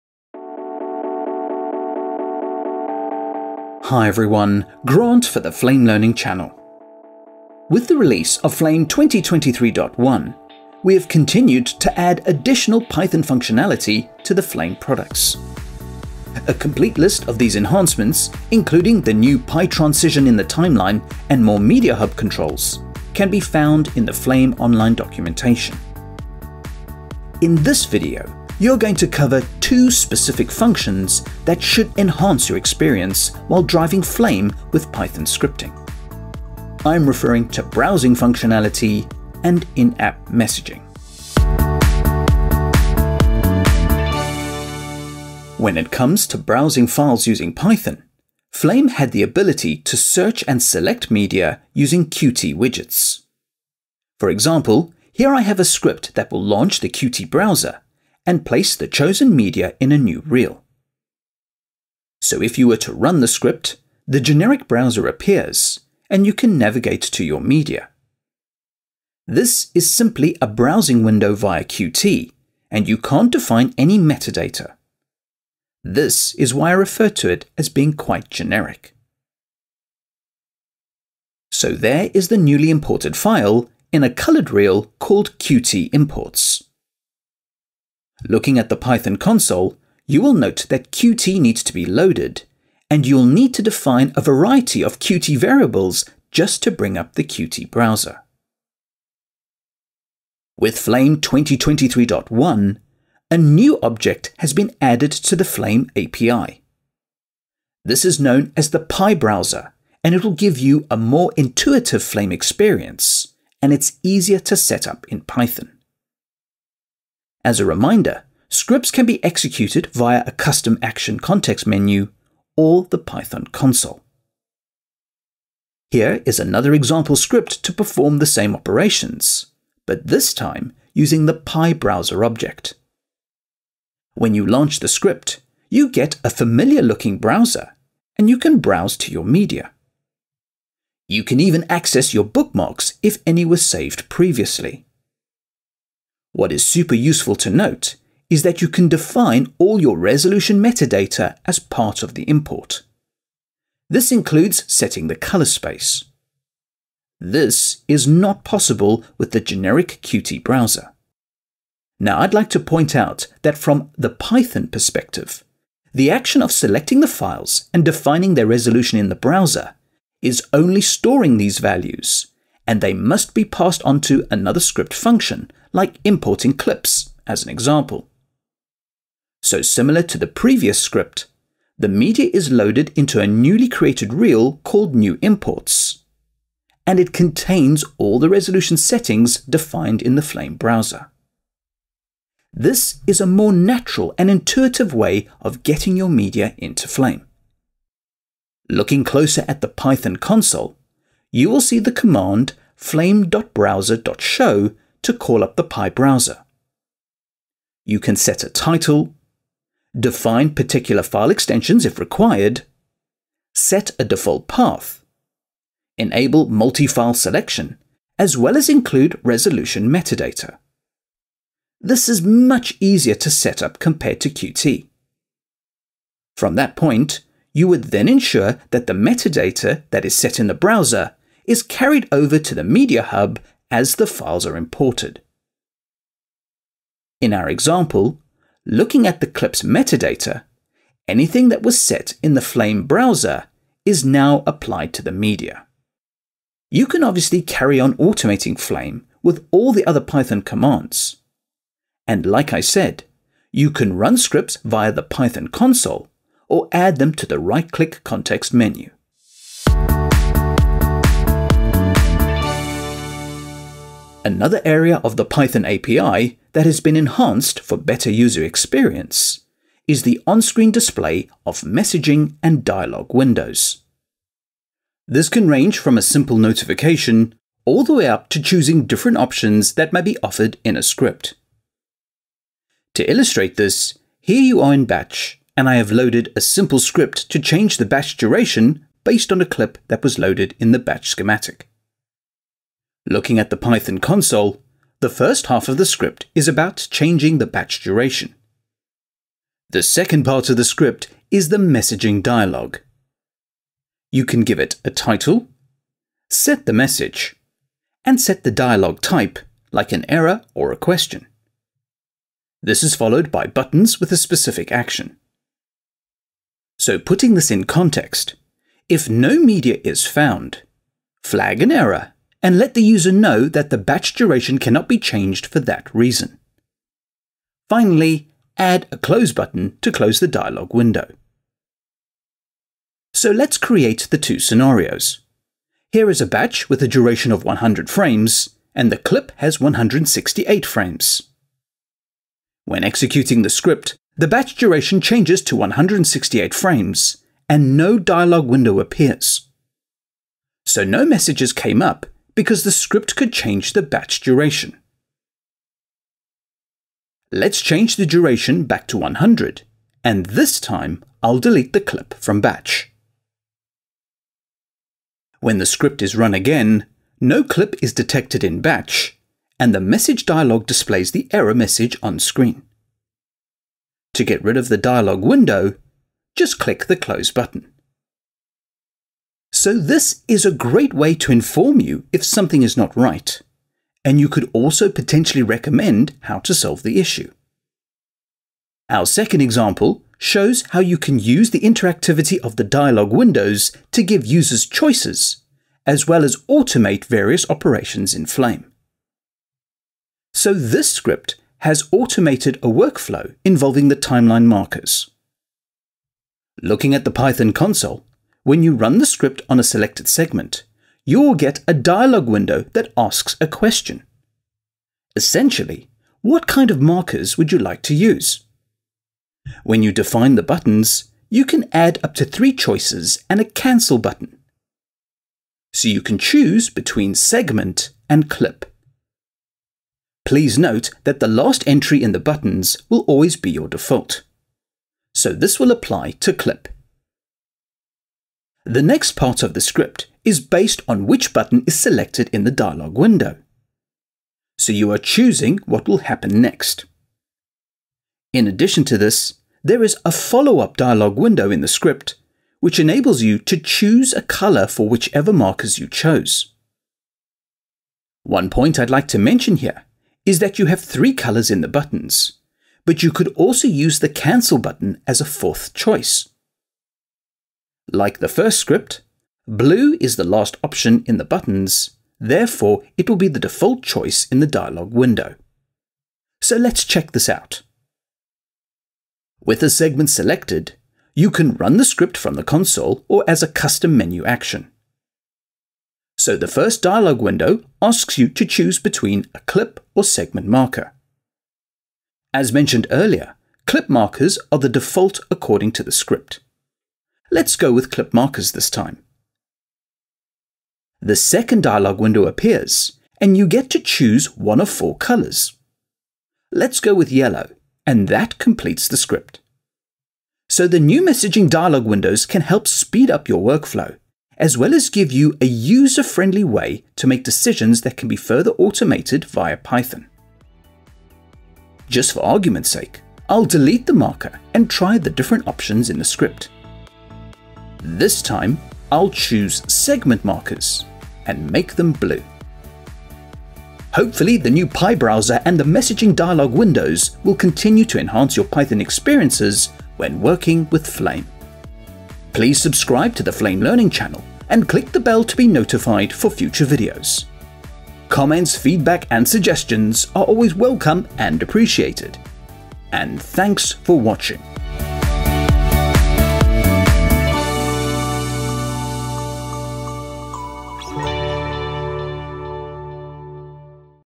Python - Browsers and Messaging - Flame 2023.1
With the release of Flame 2023.1, more Python functionality has been added to the Flame Products.   This is available via the Flame API and a full list is available on the Flame "What's New" Documentation.  This includes the new PyTransition for the Timeline and various MediaHub controls.  This video will specifically cover browsing and messaging functionality using Python.   This should enhance your user experience while driving Flame through scripting.

Level: Intermediate
Recorded in: Autodesk Flame 2023.1

If you wish to follow along, you can download the 30-day Flame trial

or if you are in education, you can sign up for a free educational license

00:00 - Introduction
00:42 - Browsing with PyBrowser
04:20 - Did you get the message?